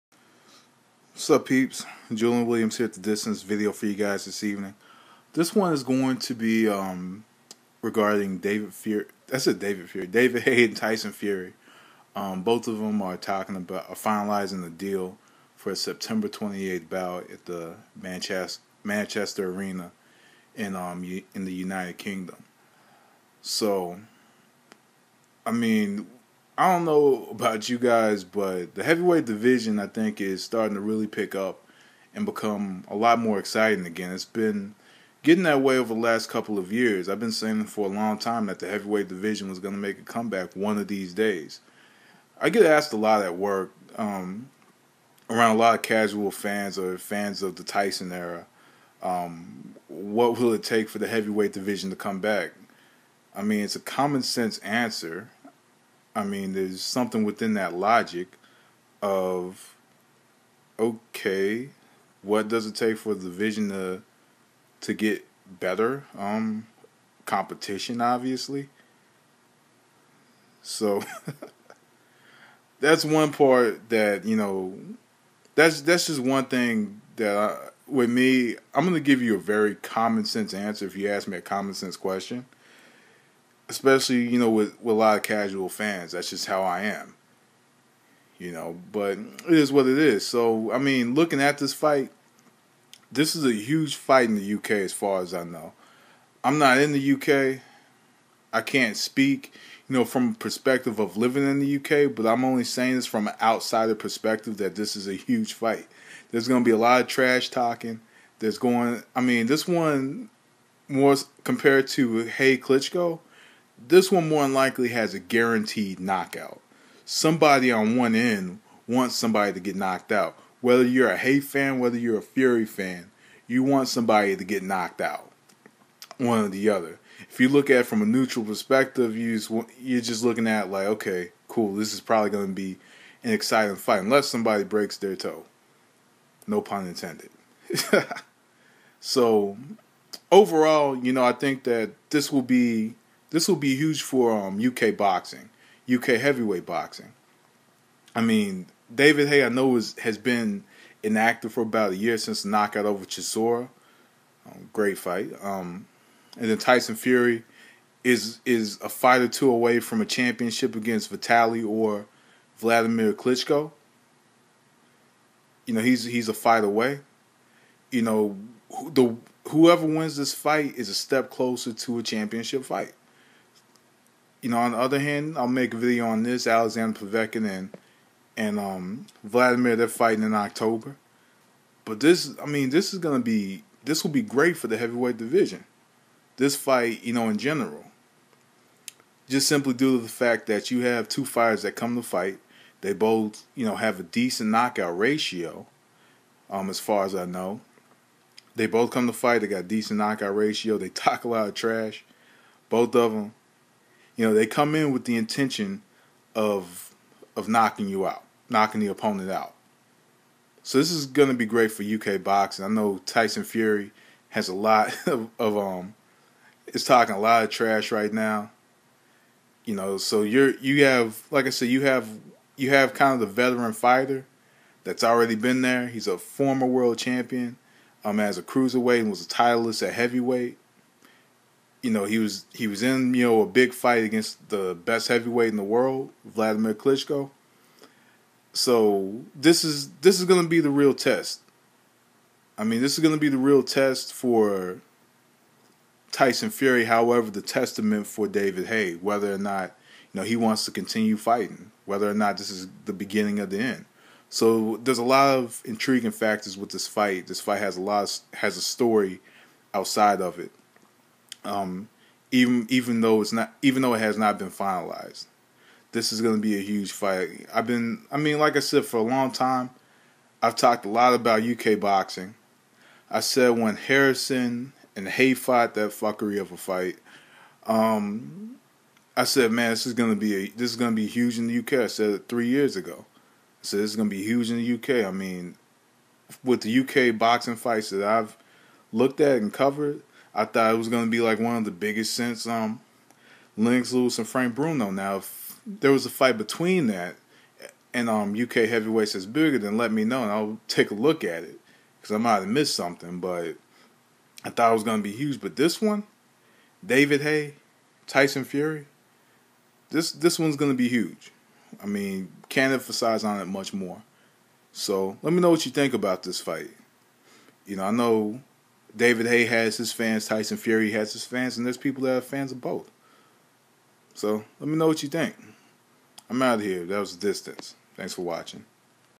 David Haye vs Tyson Fury: Almost Done
Thoughts on this potential bout